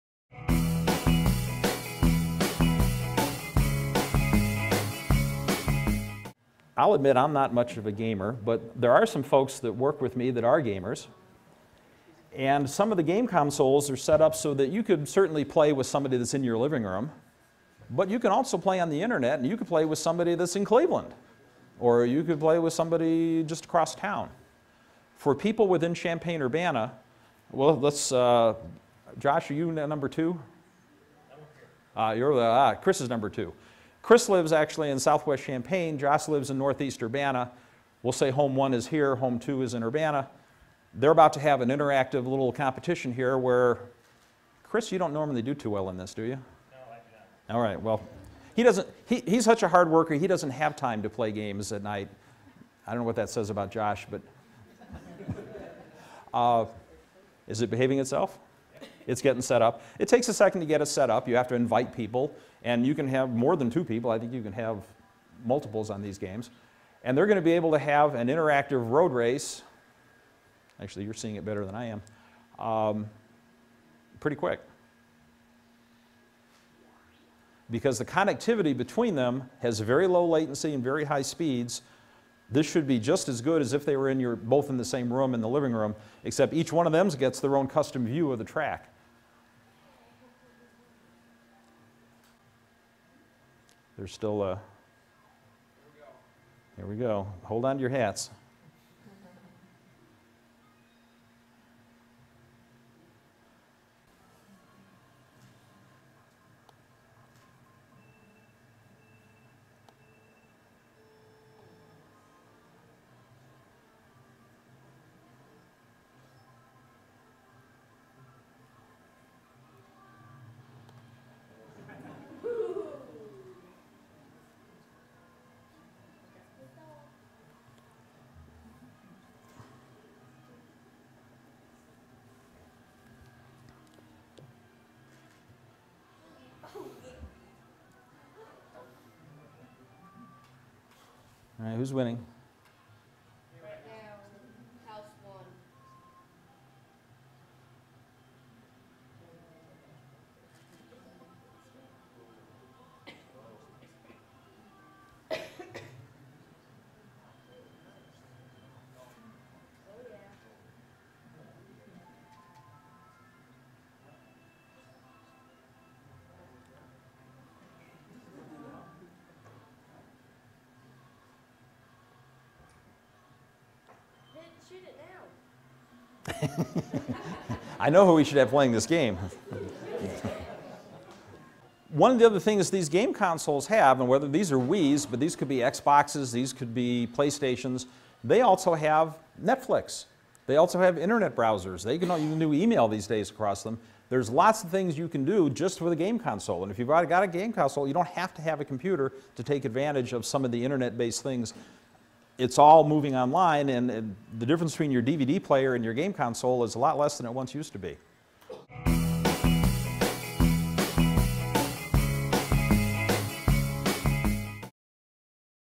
Gigabit Cross-Town Gaming over UC2B
An informational piece on how Urbana-Champaign Big Broadband subscribers can utilize gaming with the UC2B infrastructure.